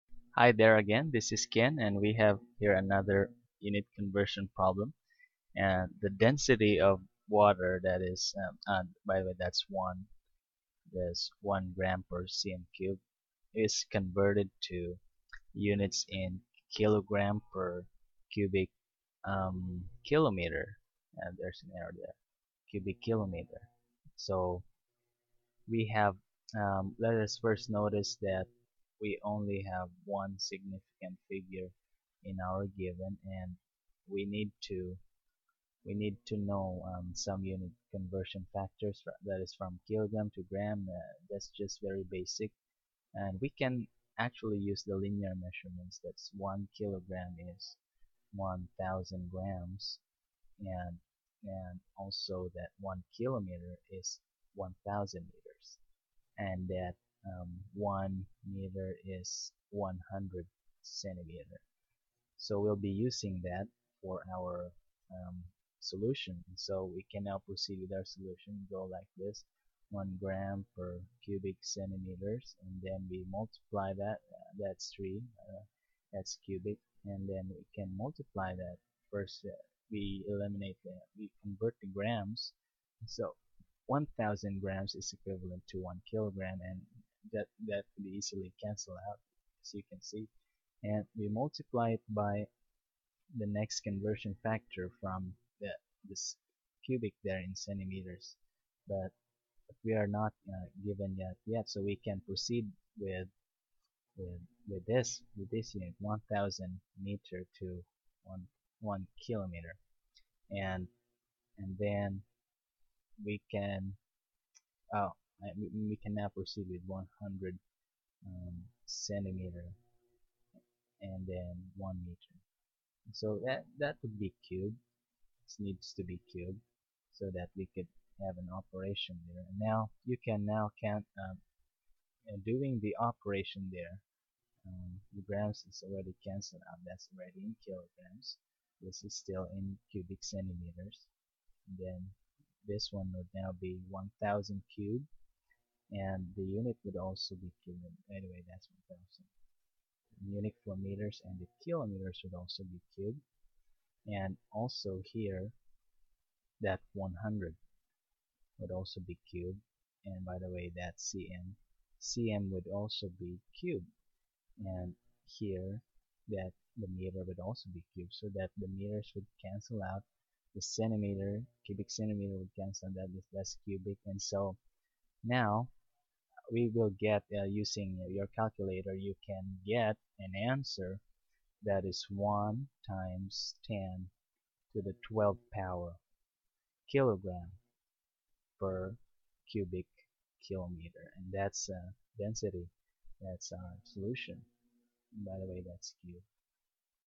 Unit conversion: density of water in g/cm^3 to kg/km^3
Unit Conversion of density measurements from g/cm^3 to kg/km^3 and answers
rounded using significant figures.